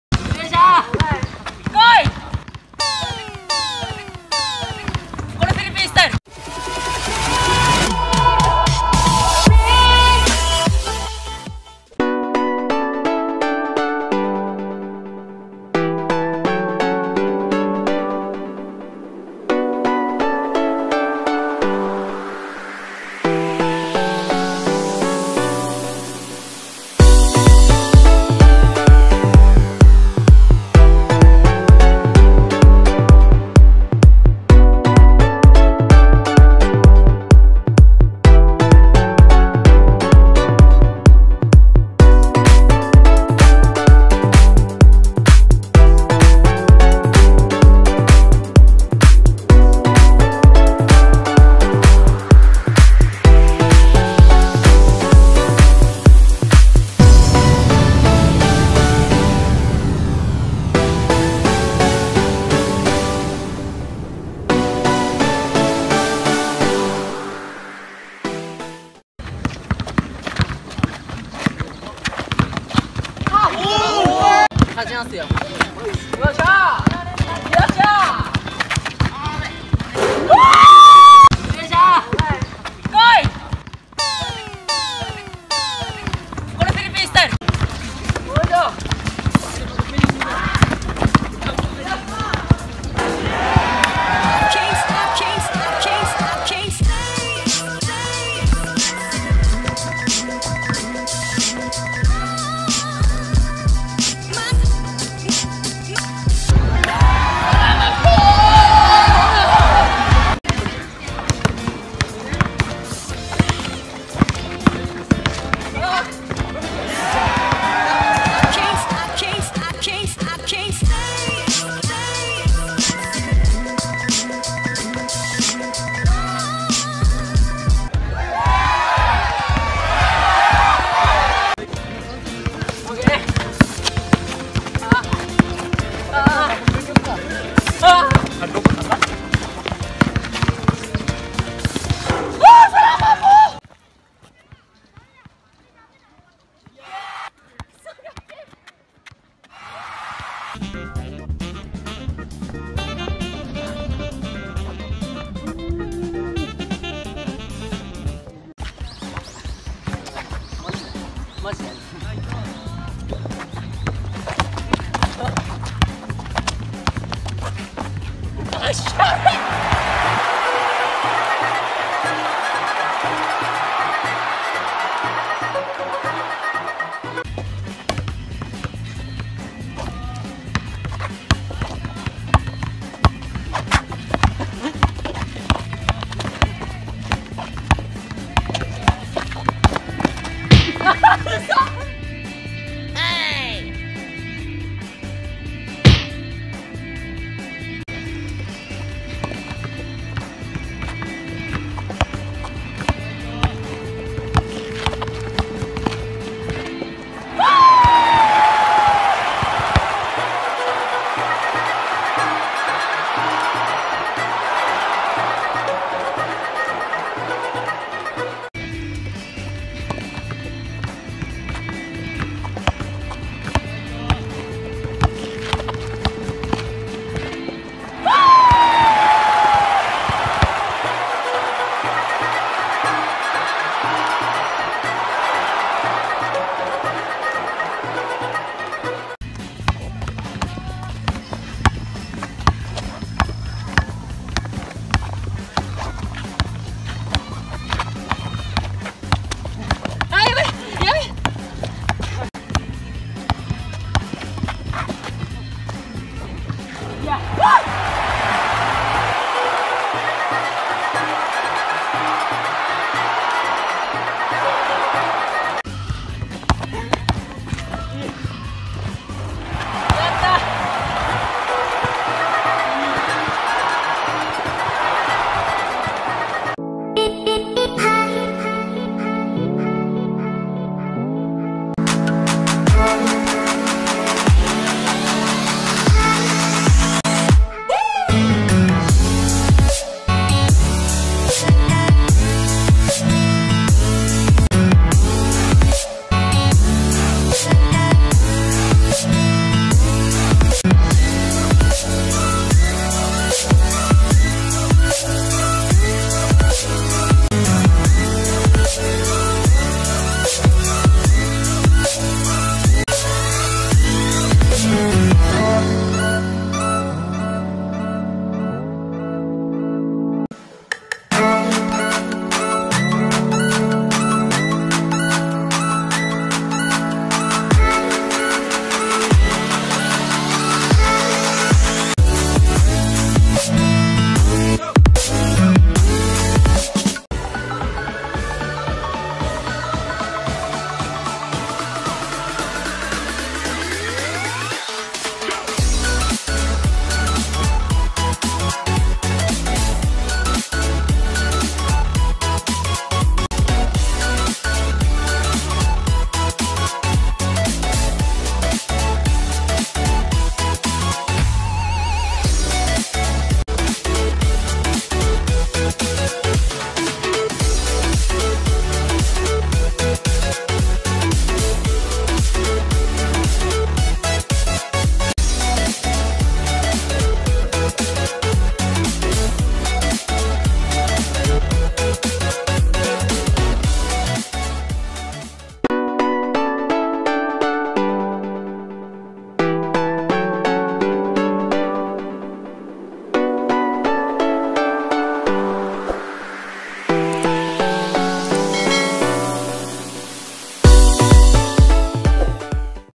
PBB Fumiya is Good at Basketball too!!! Na-amaze talaga ako!
Hindi lang pala Vlogger and PBB Housemates si FUMIYA magaling din pala sa BASKET BALL!!!

BY THE WAY CREDITS TO FUMIYA and sa mga VIDEOS NIYA!